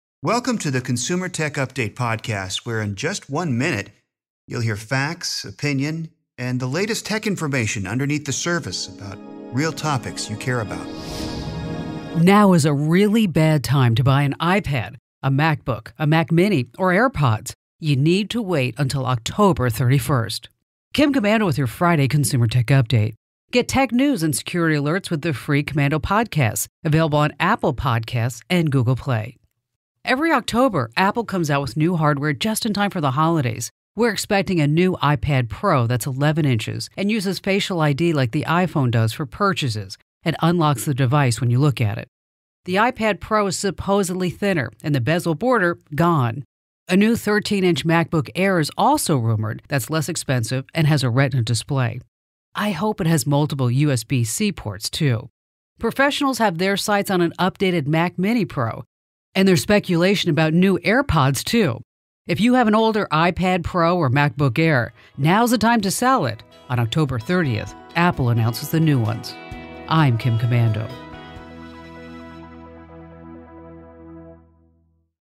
Don’t buy an iPad, Mac or AirPods... right now.
Apple comes out with new hardware closer to the holidays and this year is no exception. But what can we expect to see? In this episode of Consumer Tech Update, Kim talks about when is the best time to buy new Apple products and what new gadgets we can expect.